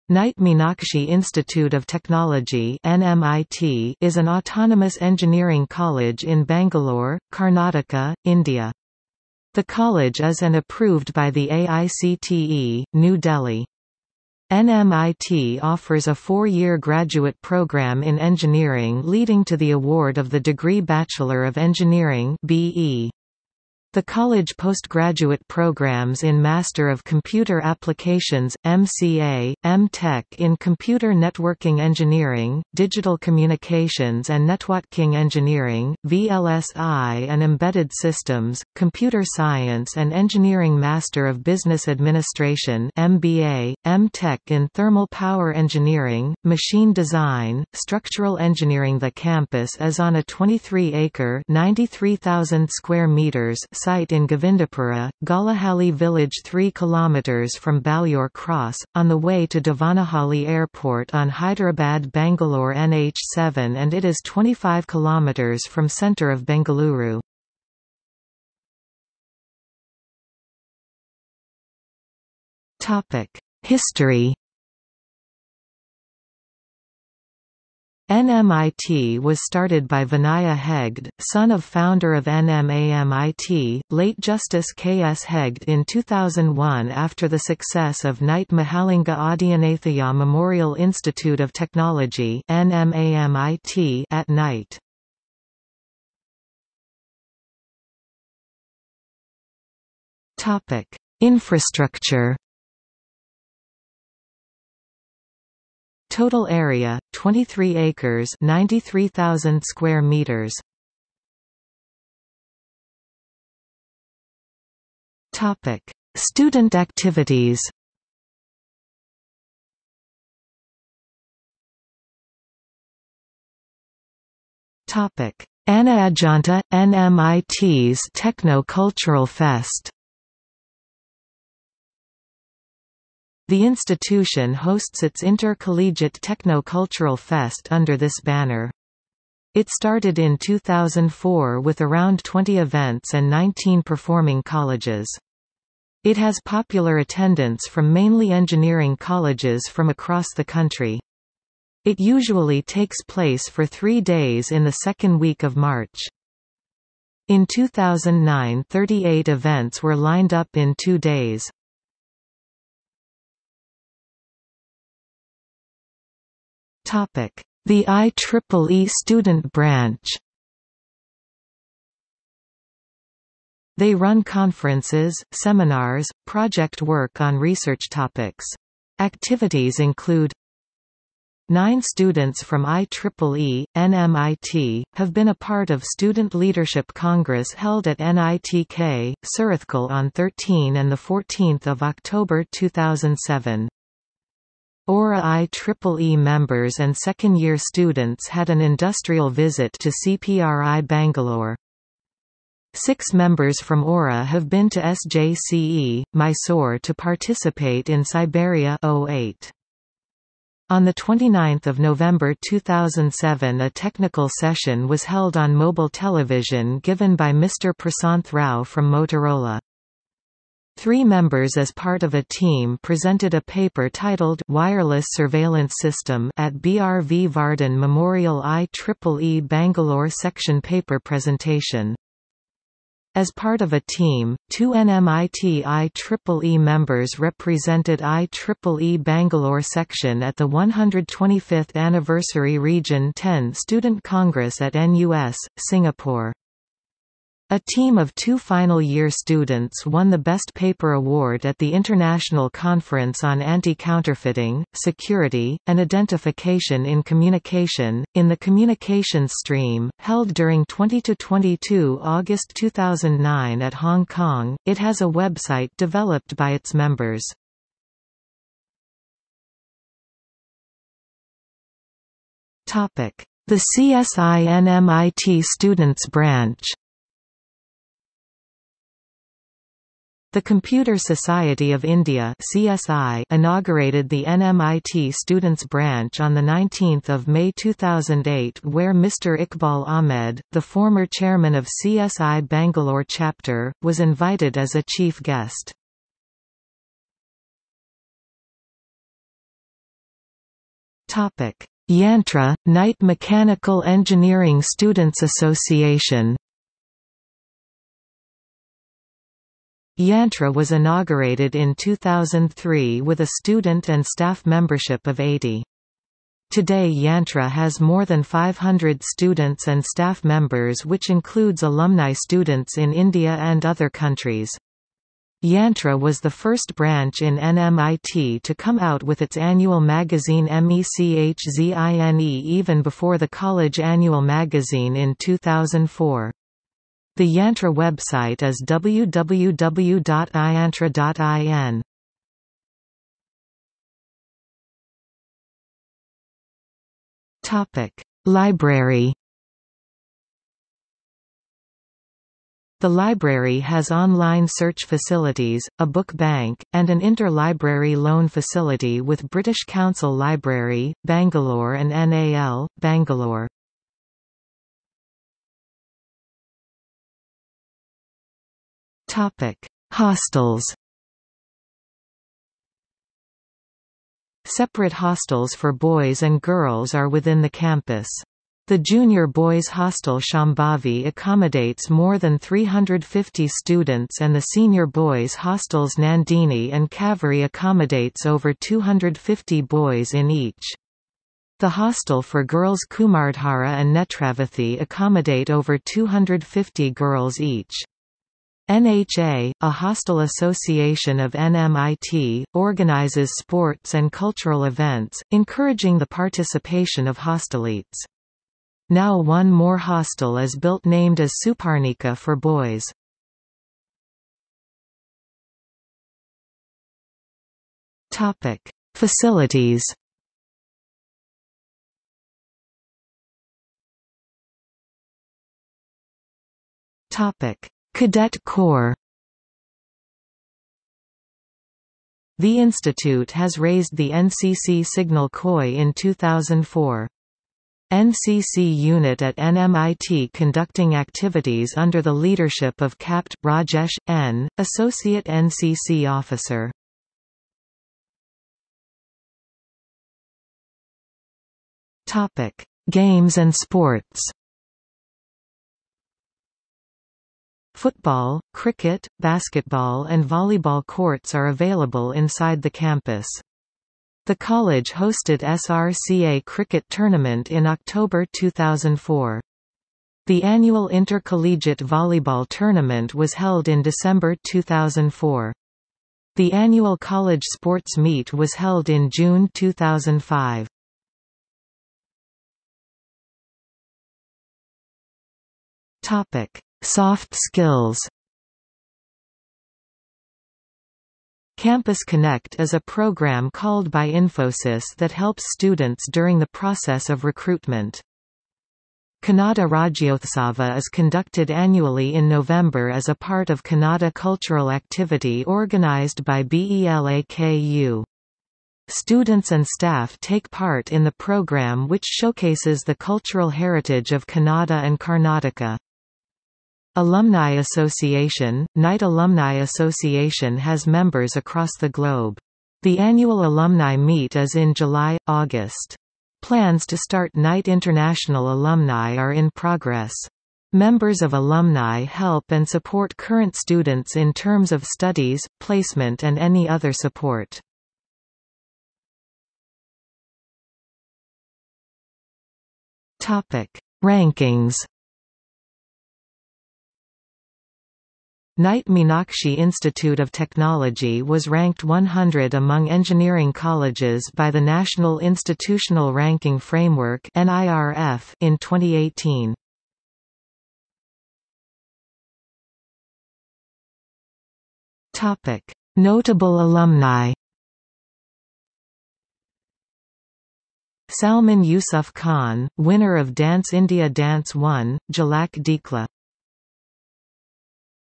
Nitte Meenakshi Institute of Technology | Wikipedia audio article
00:01:09 1 History
00:01:36 2 Infrastructure
00:01:51 3 Student activities
00:02:01 3.1 Anaadyanta - NMIT's techno-cultural fest
00:02:43 3.2 The IEEE Student Branch
00:04:29 3.3 The CSI NMIT Students' Branch
00:04:57 3.4 Yantra: Nitte Mechanical Engineering Students Association
00:05:43 4 Library
00:06:05 5 Hostels
00:06:54 6 Facilities
00:07:03 6.1 Cadet Corps
00:07:28 6.2 Games and sports
00:08:02 6.3 Soft skills
00:08:59 7 Rankings
00:09:21 8 Notable alumni

- Socrates

SUMMARY
Nitte Meenakshi Institute of Technology (NMIT) is an autonomous engineering college in Bangalore, Karnataka, India. The college is and approved by the AICTE, New Delhi. NMIT offers a four-year graduate programme in engineering leading to the award of the degree Bachelor of Engineering (B.E.). The college postgraduate programmes in

Master of Computer Applications - MCA,
M.Tech in -Computer Networking Engineering,Digital Communications &Netwotking Engineering,VLSI & Embedded Systems,Computer Science & Engineering
M.Tech in Thermal Power Engineering,Machine Design,Structural EngineeringThe campus is on a 23-acre (93,000 m2) site in Govindapura, Gollahalli village 3 km from Baglur cross, on the way to Devanahalli airport on Hyderabad-Bangalore NH 7 and it is 25 km from Centre of Bengaluru.